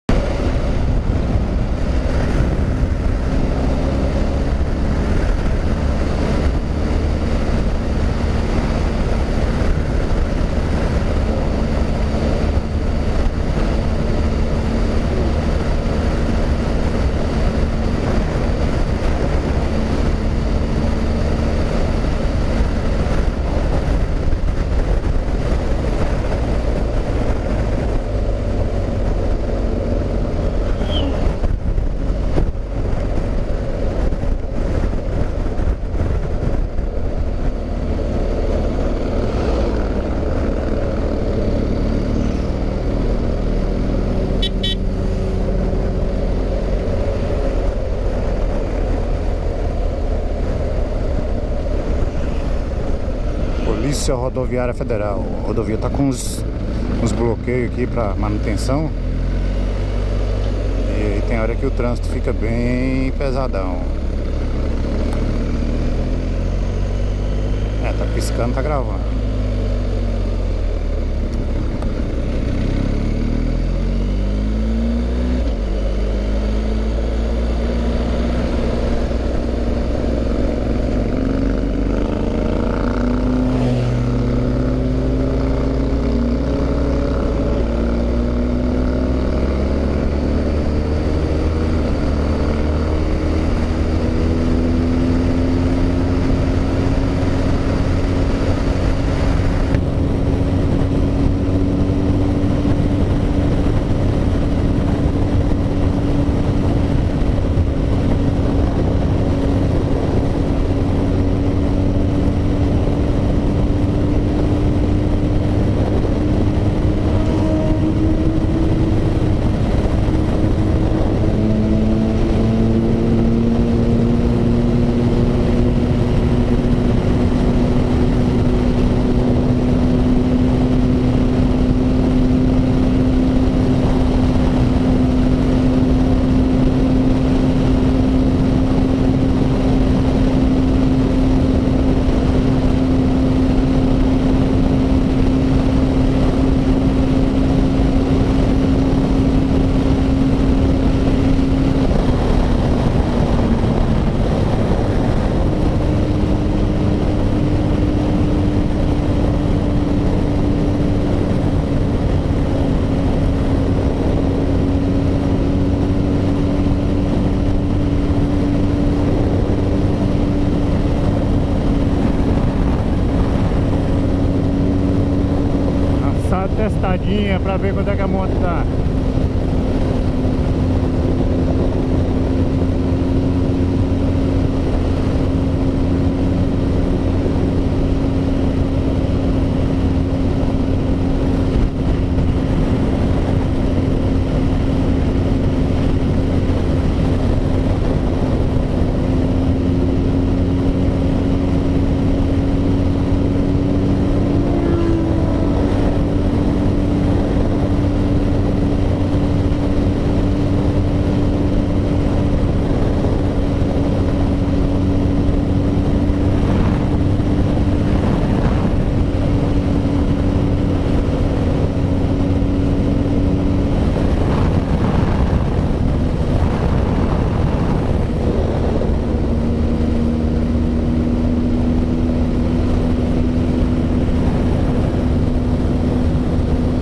BMW R1250GS ADV TRIPLE BLACK - TOP SPEED
Primeiro vez que acelerei a moto até no tudo que dá